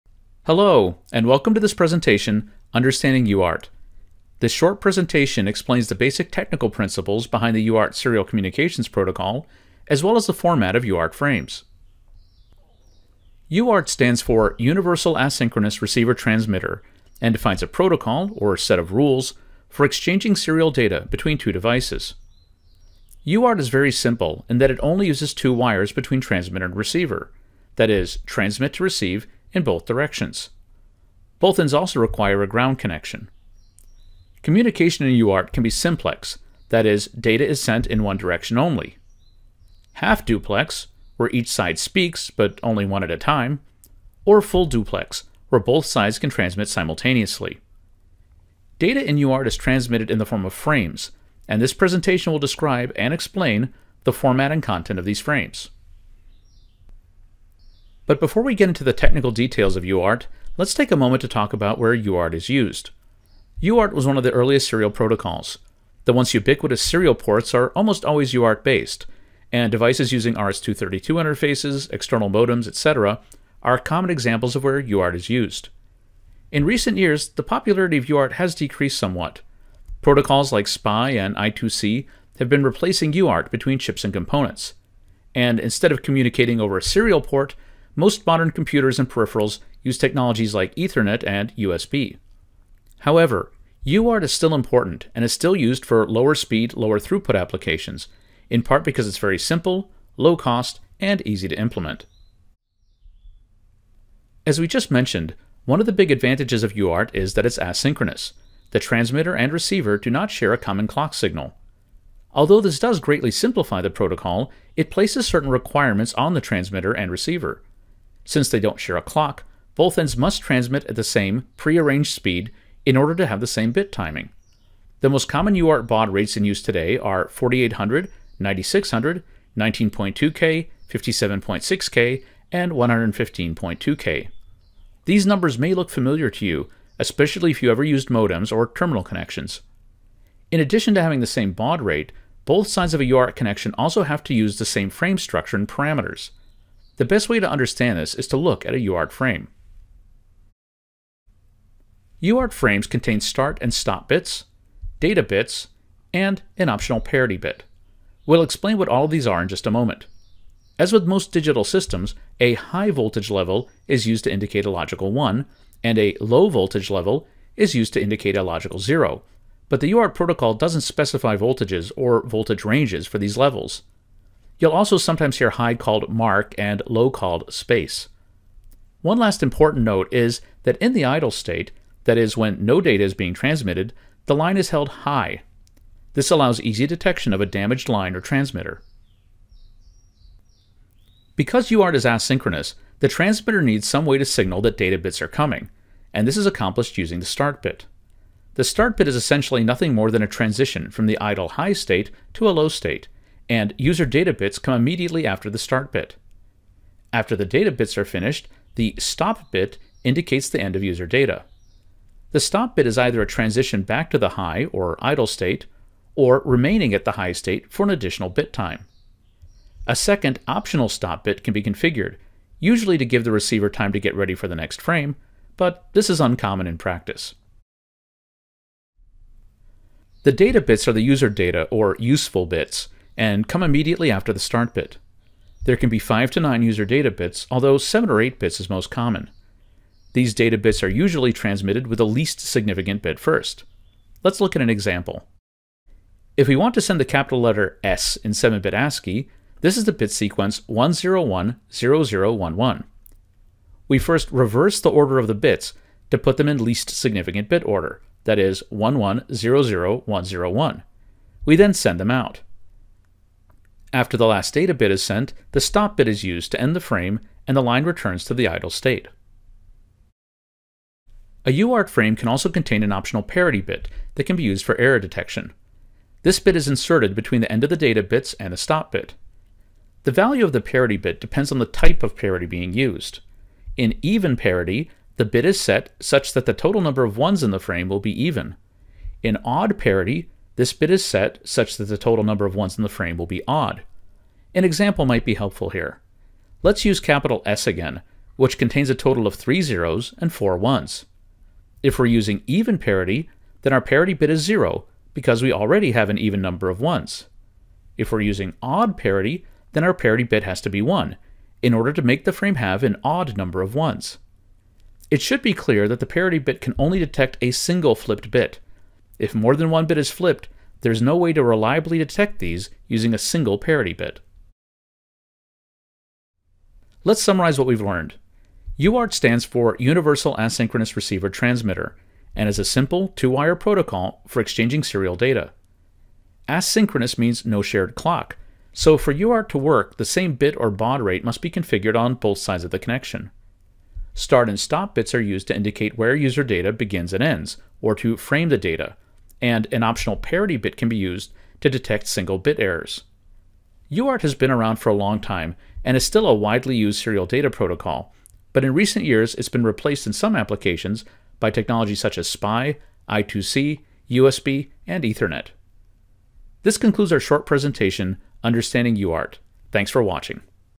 Understanding UART
This video explains the technical overview of the UART (universal asynchronous receiver/transmitter) serial protocol, including a description of the frame structure and the significance of each frame bit.